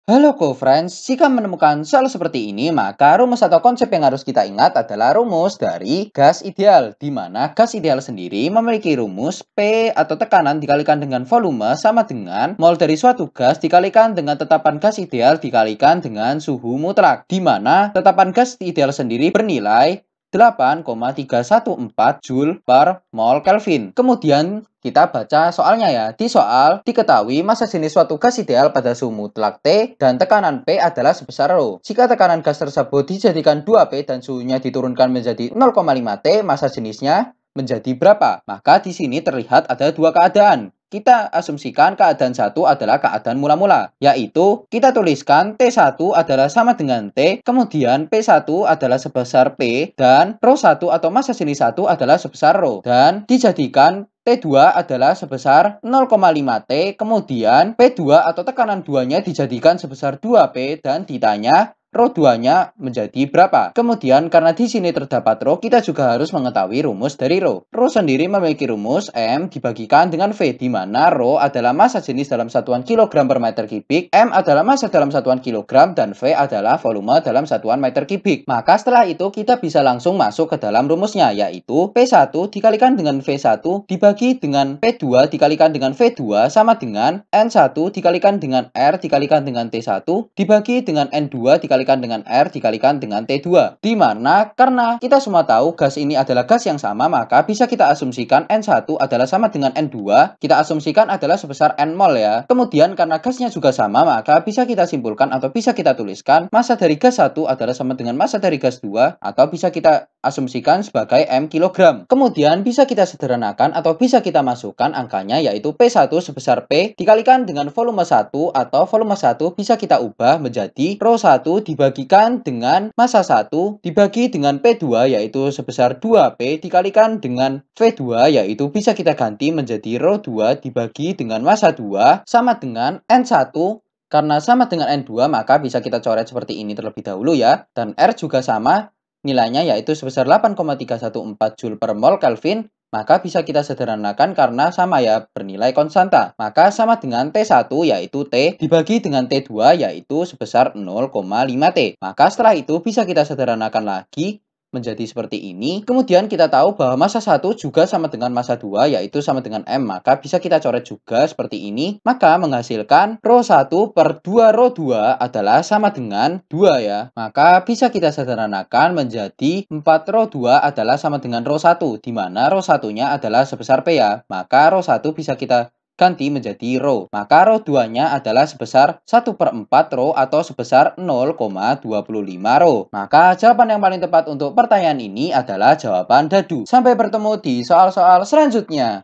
Massa jenis suatu gas ideal pada suhu mutlak T dan tekanan p adalah rho. Jika tekanan gas tersebu...
Massa jenis suatu gas ideal pada suhu mutlak T dan tekanan p adalah rho. Jika tekanan gas tersebut dijadikan 2p dan suhunya diturunkan menjadi 0,5T, massa jenis gas menjadi .

Topik atau Materi: Hukum Boyle-Gay Lussac - Teori Kinetik Gas - Termodinamika - Fisika Kelas 11 SMA -

Sumber Soal: Mudah dan Aktif Belajar Fisika SMA Kelas 11, DI, 2009 , Pusat Perbukuan Departemen Pendidikan Nasional

00:00 : Bedah Soal
01:00 : Konsep, rumus dan pengertian Hukum Boyle-Gay Lussac
01:18 : Langkah penyelesaian soal
02:09 : Jawaban akhir
02:39 : Penutup